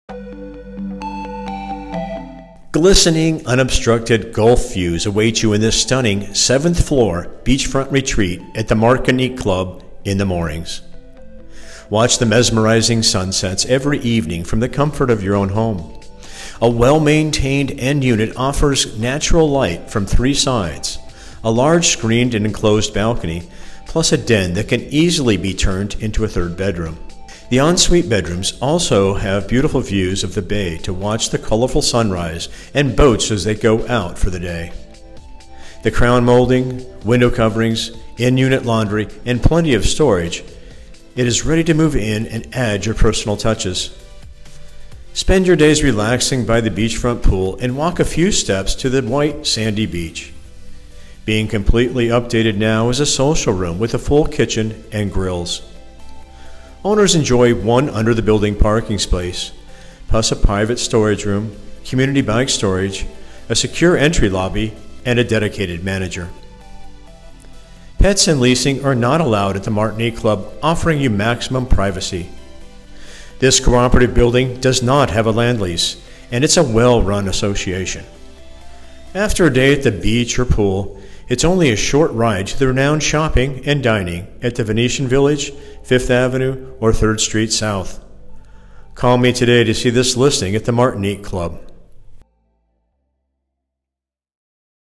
Roorda Listing | Martinique Club #701 | Naples, FL | Condominiums for Sale
Glistening unobstructed Gulf views await you in this stunning seventh floor beachfront retreat at Martinique Club in The Moorings.

Watch the mesmerizing sunsets every evening from the comfort of your home.

This well-maintained end-unit offers natural light from three sides, a large screened and enclosed balcony, plus a den that can easily be turned into a third bedroom.

The en-suite bedrooms also have beautiful views of the Bay to watch the colorful sunrise and boats as they go out for the day.

With crown molding, window coverings, in-unit laundry, and plenty of storage, it is ready to move in and add your personal touches.

Spend your days relaxing by the beachfront pool and walk a few steps to the white sandy beach.

Being completely updated now is a social room with a full kitchen and grills.

Owners enjoy one under-building parking space, plus a private storage room, community bike storage, a secure entry lobby and dedicated manager.

Pets and leasing are not allowed at Martinique Club offering you maximum privacy.

This cooperative building has no land lease and a well-run association.

After a day at the beach or pool, it’s only a short ride to the renowned shopping & dining at Venetian Village, Fifth Avenue or Third Street South.